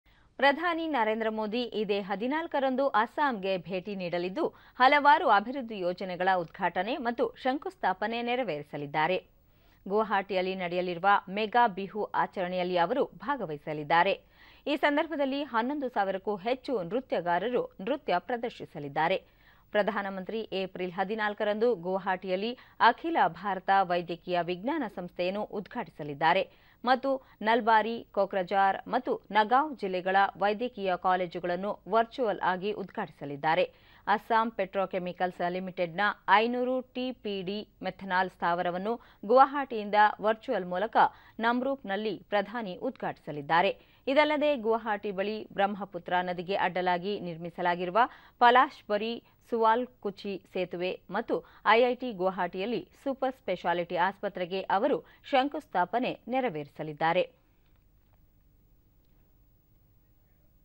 ಪ್ರಧಾನಿ ನರೇಂದ್ರ ಮೋದಿ ಇದೇ 14 ರಂದು ಅಸ್ಸಾಂಗೆ ಭೇಟಿ ; ಹಲವು ಅಭಿವೃದ್ಧಿ ಯೋಜನೆಗಳ ಉದ್ಘಾಟನೆ ಮತ್ತು ಶಂಕುಸ್ಥಾಪನೆ
ಪ್ರಧಾನಿ ನರೇಂದ್ರ ಮೋದಿ  ಇದೇ 14 ರಂದು  ಅಸ್ಸಾಂಗೆ ಭೇಟಿ ನೀಡಲಿದ್ದು, ಹಲವಾರು ಅಭಿವೃದ್ಧಿ ಯೋಜನೆಗಳ ಉದ್ಘಾಟನೆ ಮತ್ತು ಶಂಕುಸ್ಥಾಪನೆ ನೆರವೇರಿಸಲಿದ್ದಾರೆ.
ಗುವಾಹಟಿಯಲ್ಲಿ ನಡೆಯಲಿರುವ  ಮೆಗಾ ಬಿಹು ಆಚರಣೆಯಲ್ಲಿ ಅವರು  ಭಾಗವಹಿಸಲಿದ್ದಾರೆ.
ಈ ಸಂದರ್ಭದಲ್ಲಿ  11,000 ಕ್ಕೂ ಹೆಚ್ಚು  ನೃತ್ಯಗಾರರು  ನೃತ್ಯ ಪ್ರದರ್ಶಿಸಲಿದ್ದಾರೆ.
ಪ್ರಧಾನಮಂತ್ರಿ  ಏಪ್ರಿಲ್ 14 ರಂದು  ಗುವಾಹಟಿಯಲ್ಲಿ  ಅಖಿಲ ಭಾರತ ವೈದ್ಯಕೀಯ ವಿಜ್ಞಾನ ಸಂಸ್ಥೆಯನ್ನು ಉದ್ಘಾಟಿಸಲಿದ್ದಾರೆ ಮತ್ತು ನಲ್ಬಾರಿ, ಕೋಕ್ರಜಾರ್ ಮತ್ತು ನಗಾಂವ್ ಜಿಲ್ಲೆಗಳ ವೈದ್ಯಕೀಯ ಕಾಲೇಜುಗಳನ್ನು ವರ್ಚುವಲ್ ಆಗಿ ಉದ್ಘಾಟಿಸಲಿದ್ದಾರೆ . ಅಸ್ಸಾಂ  ಪೆಟ್ರೋ-ಕೆಮಿಕಲ್ಸ್  ಲಿಮಿಟೆಡ್ ನ  500  ಟಿ ಪಿ ಡಿ  ಮೆಥನಾಲ್ ಸ್ಥಾವರವನ್ನು ಗುವಾಹಟಿಯಿಂದ ವರ್ಚುವಲ್ ಮೂಲಕ  ನಮ್ರೂಪ್ ನಲ್ಲಿ  ಪ್ರಧಾನಿ ಉದ್ಘಾಟಿಸಲಿದ್ದಾರೆ. ಇದಲ್ಲದೆ, ಗುವಾಹಟಿ ಬಳಿ  ಬ್ರಹ್ಮಪುತ್ರಾ ನದಿಗೆ  ಅಡ್ಡಲಾಗಿ  ನಿರ್ಮಿಸಲಾಗಿರುವ  ಪಲಾಶ್ಬರಿ-ಸುವಾಲ್ಕುಚಿ ಸೇತುವೆ ಮತ್ತು ಐಐಟಿ ಗುವಾಹಟಿಯಲ್ಲಿ  ಸೂಪರ್ ಸ್ಪೆಷಾಲಿಟಿ ಆಸ್ಪತ್ರೆಗೆ ಅವರು ಶಂಕುಸ್ಥಾಪನೆ ನೆರವೇರಿಸಲಿದ್ದಾರೆ.